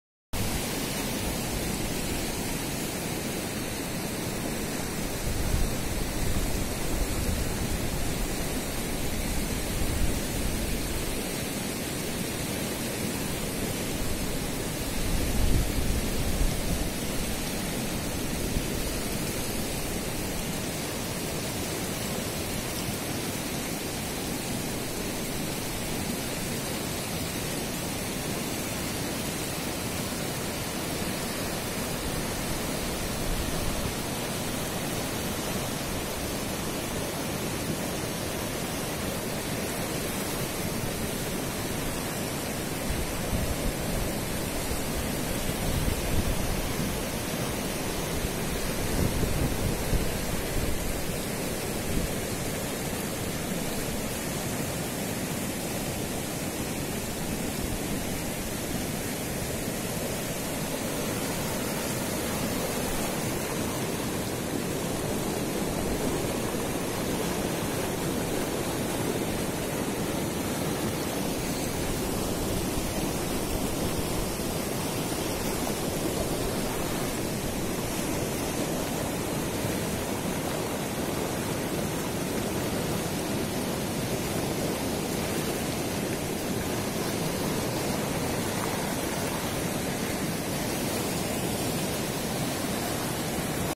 Redmi Note 7 pro video shot in 4k at Chitkul Sample 2
Used zhiyun smooth q for external stabilization.